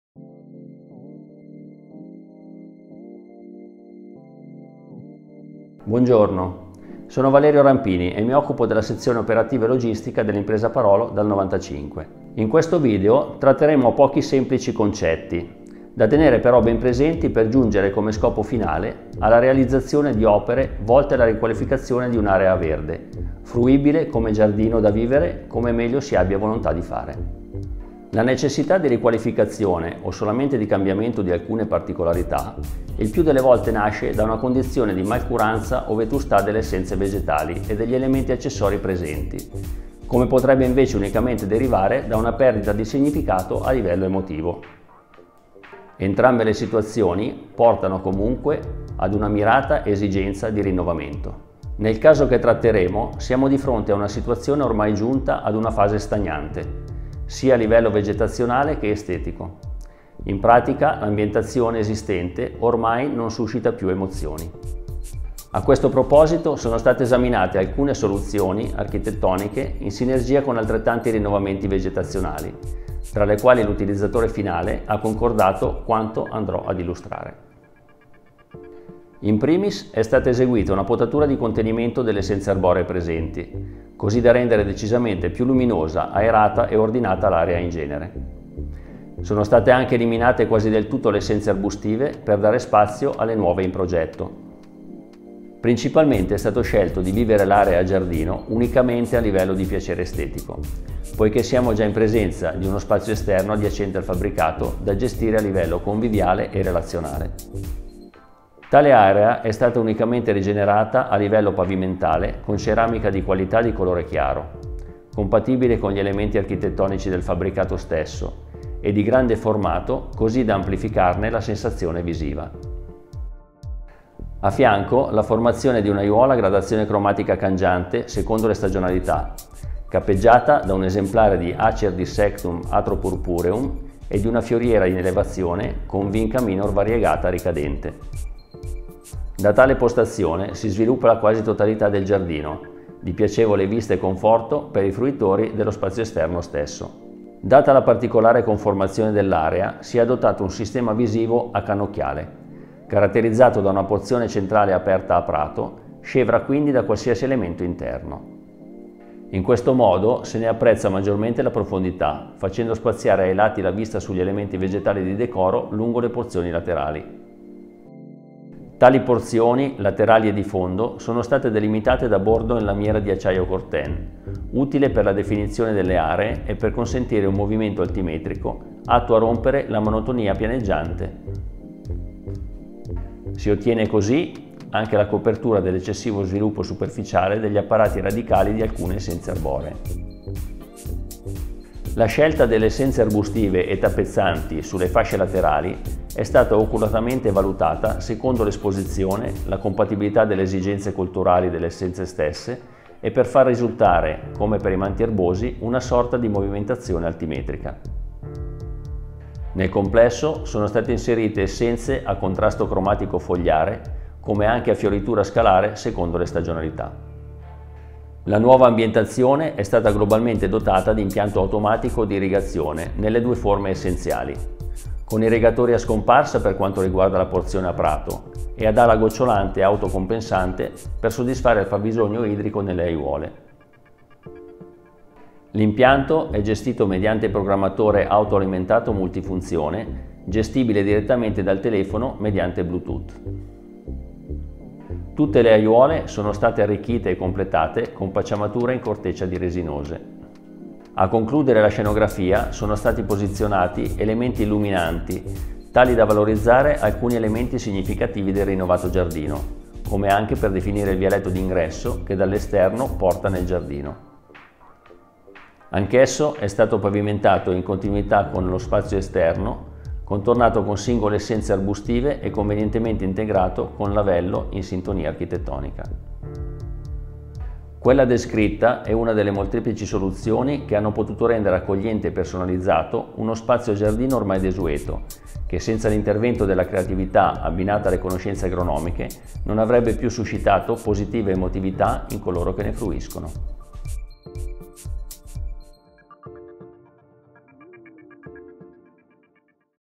Parolo Impresa & Servizi - Riqualificazione di un'area verde
Nel video il Geometra - Perito Agrario Sig. Rampini Valerio, spiega approfonditamente le opere di riqualificazione eseguite in un giardino privato.